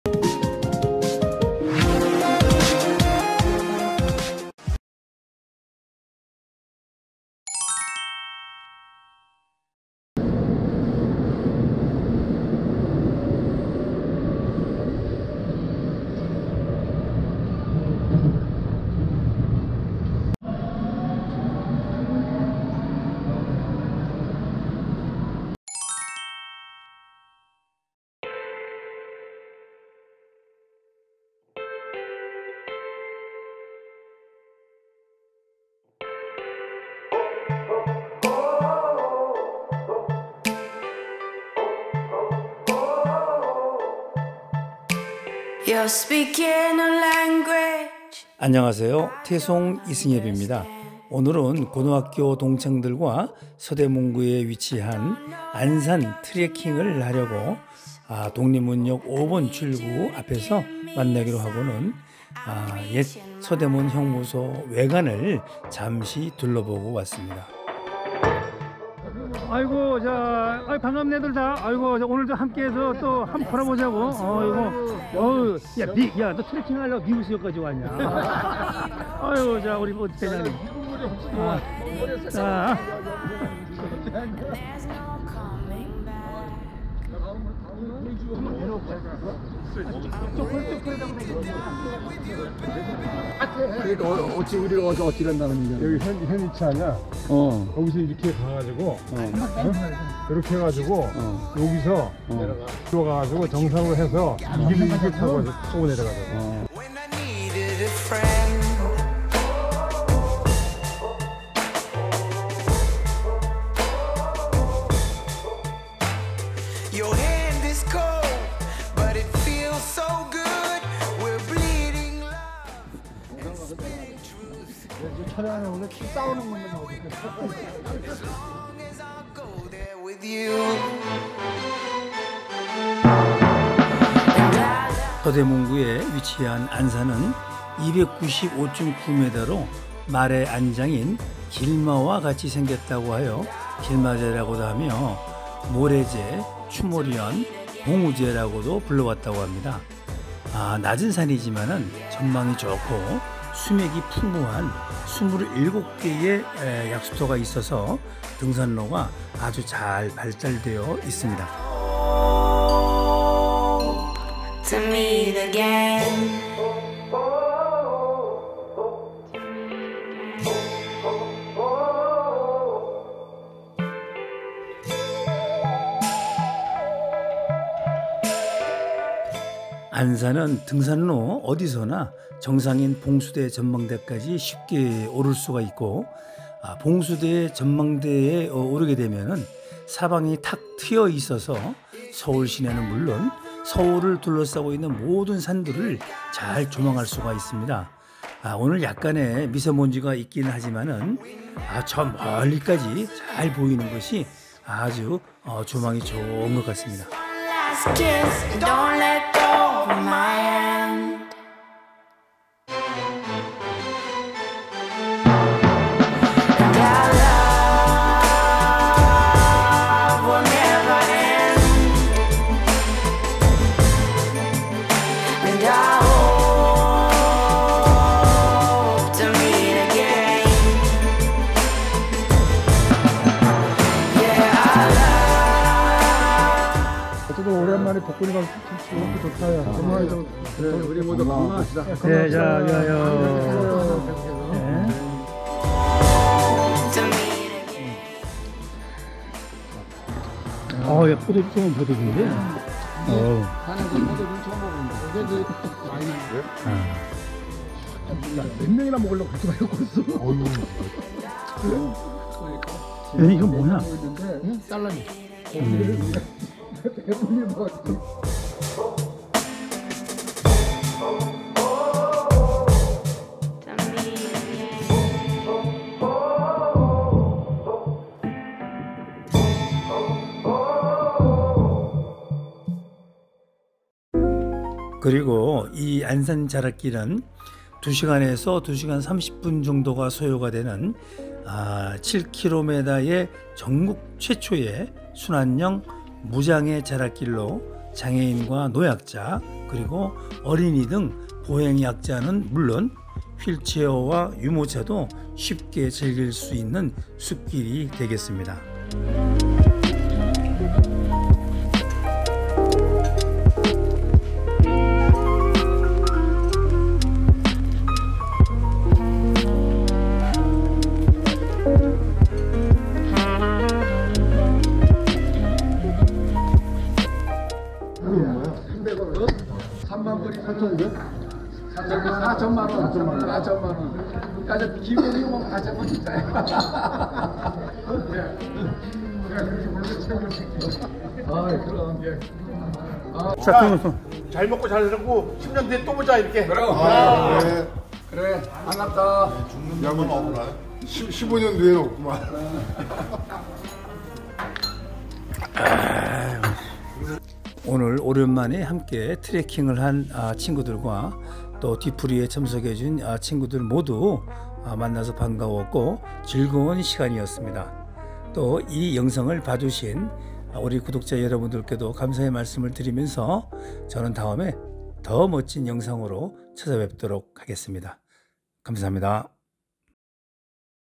[4K] 고교 동창들과 안산 트래킹
고등학교 동창들과 함께 서대문구에 위치한 안산의 자락길 일부와 정상인 봉수대 전망대로 오르는 가벼운 트래킹을 하는 즐거운 시간을 담아 봤습니다.